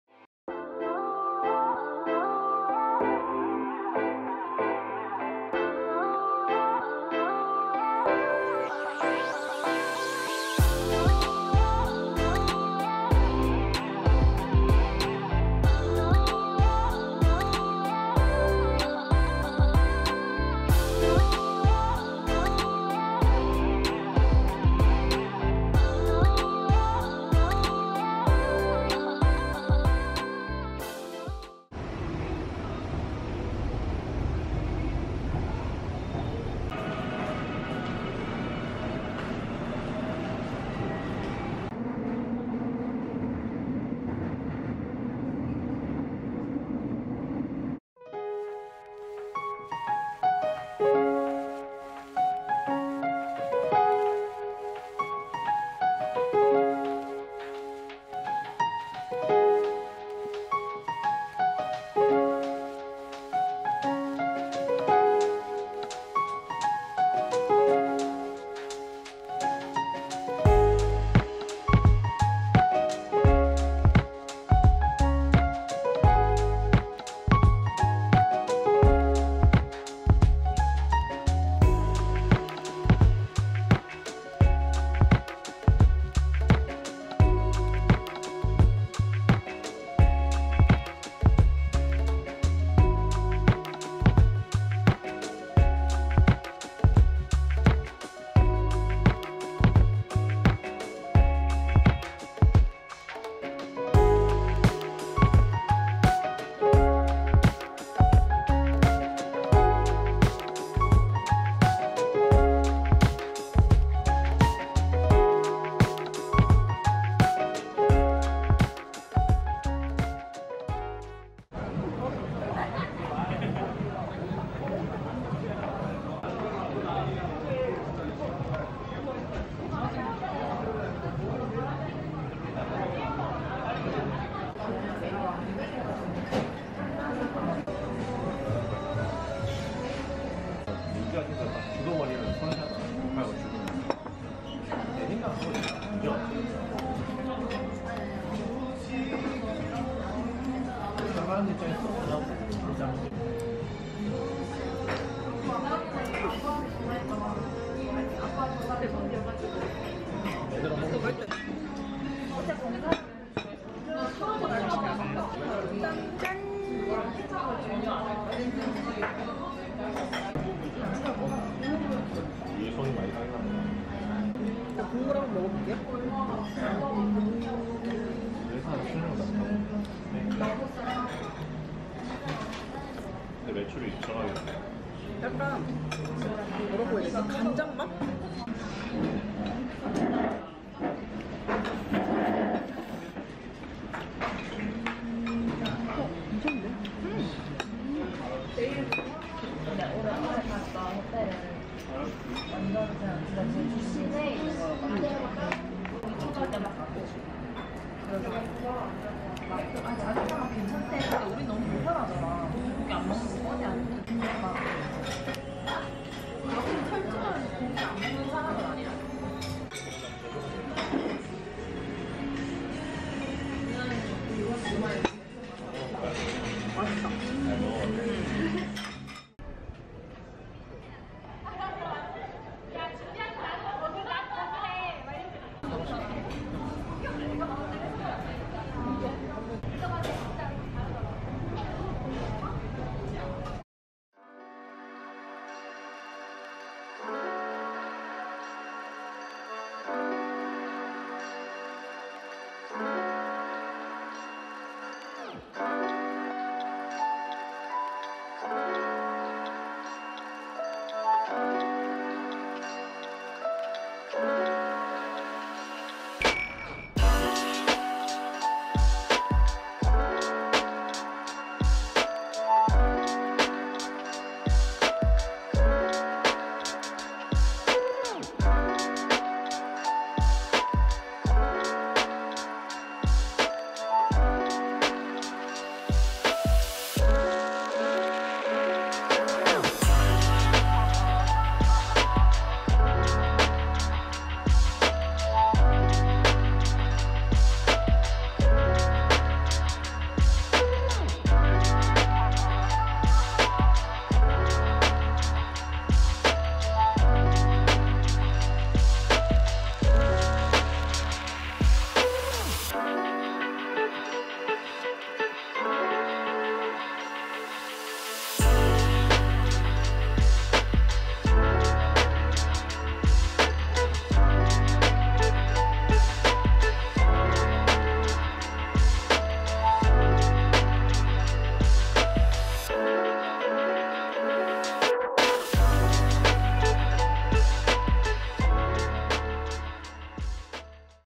[ENG/JPN] 샤로수길 데이트 : 면화당 | 아열대
안녕하세요. 유진입니다.
절친동생과 샤로수길데이트했어요.
오늘도 브이로그 보러 와주셔서 감사합니다:)

📷 cannon mark2 g7x
💻 Adobe Premiere Pro CC